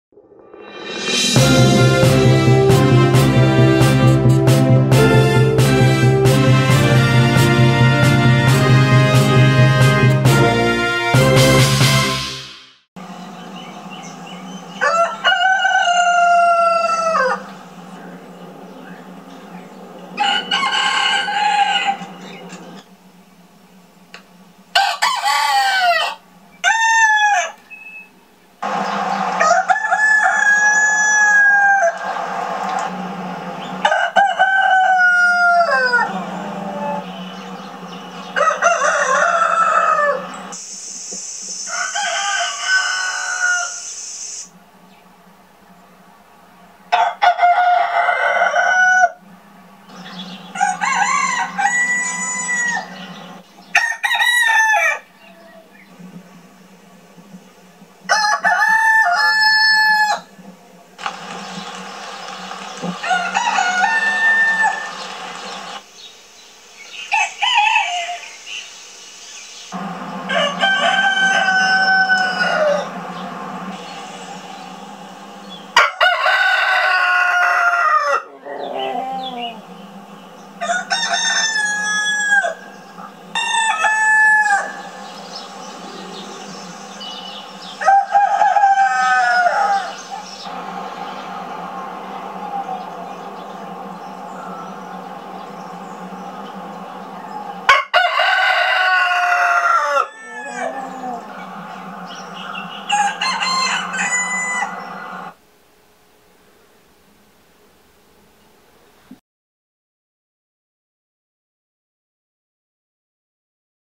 chupa's favorite video
Chupa loves this video.  Rooster Crowing Compilation Plus by YANG Edwin has had almost 30 million views.